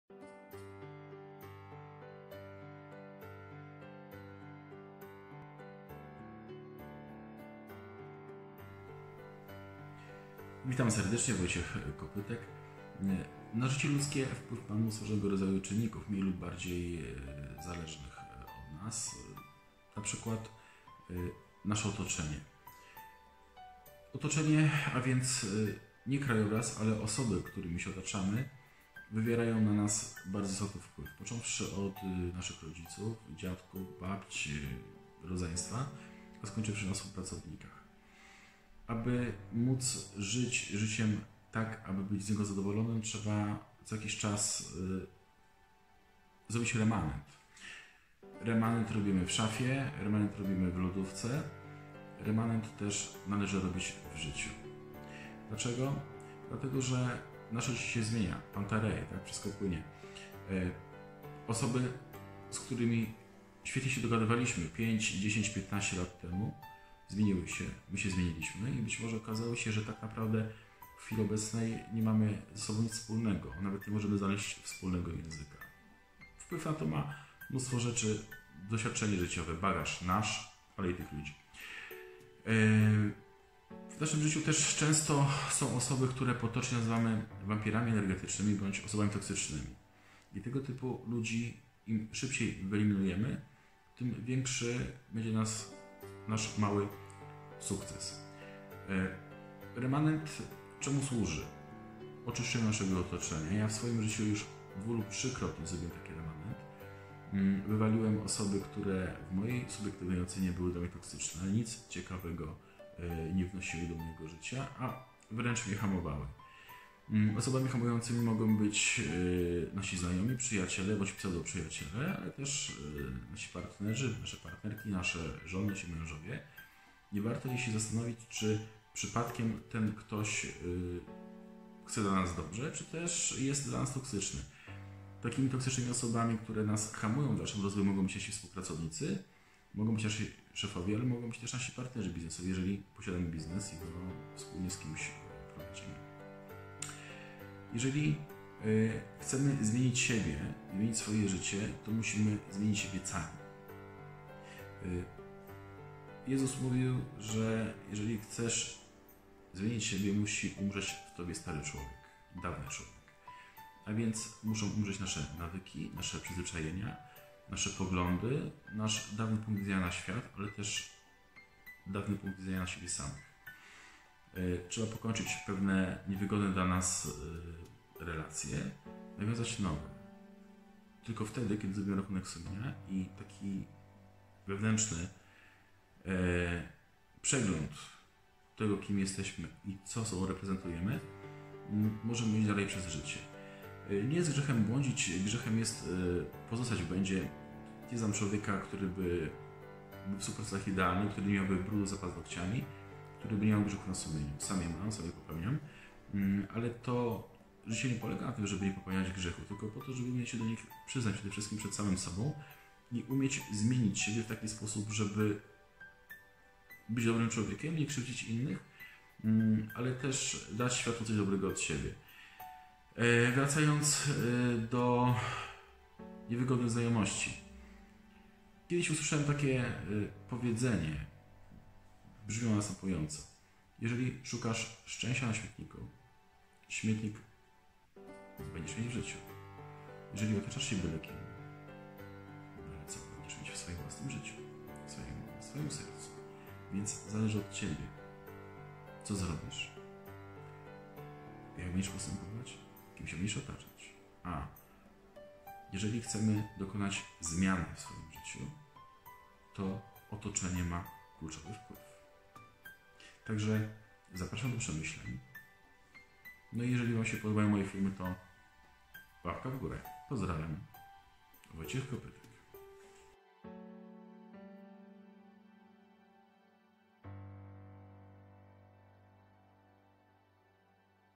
Zmiana sposobu życia
Kilka słów na temat zmiany sposobu życia.

Na nasze życie ma wpływ mnóstwo czynników, zarówno od nas niezależnych jak i zależnych.

Jeśli uważamy, że nasze dotychczasowe życie nam nie odpowiada, powinniśmy dążyć do zmiany tego, co zmienić możemy, zignorować to, czego zmienić nie możemy i skutecznie odróżniać jedno od drugiego, aby nie tracić czasu i nie popadać we frustrację.

Ważne, aby żyć zgodnie z własnym sumieniem i normami etycznymi, ich złamanie bardzo często prowadzi do opłakanych skutków w zdrowiu psychicznym.

To, co zmienić możemy, to nasze nawyki, sposób myślenia o sobie, otoczenie, czyli ludzie którymi się otaczamy, sposób operowania słowem oraz wiele innych kwestii.

Zmiana tych kilku rzeczy wymaga siły psychicznej i wytrwałości, jednak warto rozpocząć drogę do zmiany, na jej końcu czeka nowa jakość życia i być może Ty jako nowy, odmieniony człowiek.

Wojciech Kopytek